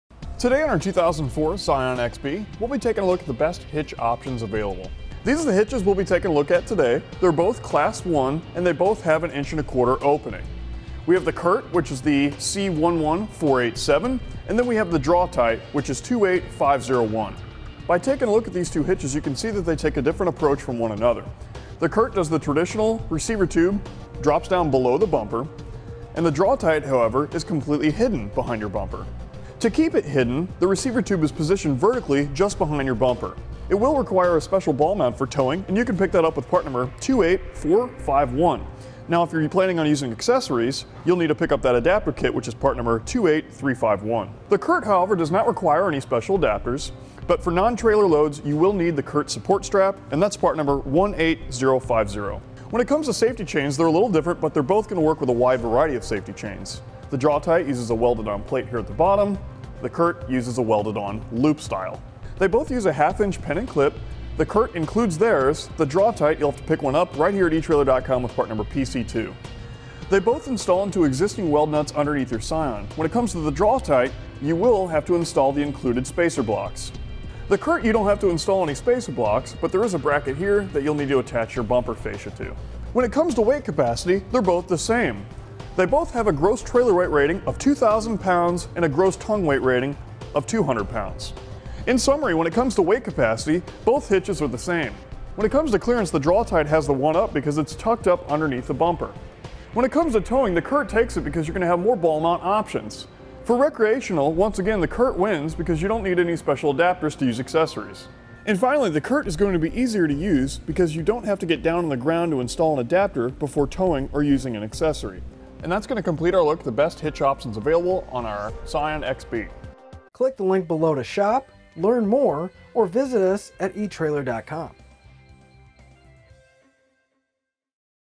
etrailer | Best 2004 Scion xB Hitch Options
Today on our 2004 Scion XB, we'll be taking a look at the best hitch options available. These are the hitches we'll be taking a look at today. They are both Class I, and then both have an inch and a quarter opening. We have the Curt, which is the C11487, and then we have the DrawTite, which is 28501. By taking a look at these two hitches, you can see that they take a different approach from one another. The Curt does the traditional receiver tube, drops down below the bumper. The DrawTite, however, is completely hidden behind your bumper.

To keep it hidden, the receiver tube is positioned vertically just behind your bumper. It will require a special ball mount for towing, and you can pick that up with part number 28451. Now, if you're planning on using accessories, you'll need to pick up that adaptor kit, which is part number 28351. The Curt, however, does not require any special adapters, but for non-trailer loads, you will need the Curt support strap, and that's part number 18050. When it comes to safety chains, they're a little different, but they're both going to work with a wide variety of safety chains. The DrawTite uses a welded on plate here at the bottom.

The Curt uses a welded on loop style. They both use a half inch pin and clip. The Curt includes theirs, the DrawTite, you'll have to pick one up, right here at eTrailer.com with part number PC2. They both install into existing weld nuts underneath your Scion. When it comes to the DrawTite, you will have to install the included spacer blocks. The Curt, you don't have to install any spacer blocks, but there is a bracket here, that you'll need to attach your bumper fascia too. When it comes to weight capacity, they're both the same.

They both have a gross trailer weight rating of 2,000 pounds, and a gross tongue weight rating of 200 pounds. In summary, when it comes to weight capacity, both hitches are the same. When it comes to clearance, the DrawTite has the one-up, because it's tucked up underneath the bumper. When it comes to towing, the Curt takes it, because you're going to have more ball mount options. For recreational, once again the Curt wins, because you don't need any special adapters to use accessories. Finally, the Curt is going to be easier to use because you don't have to get down on the ground to install and adapter before towing, or using an accessory. That's going to complete our look at the best hitches options available on our Scion XB..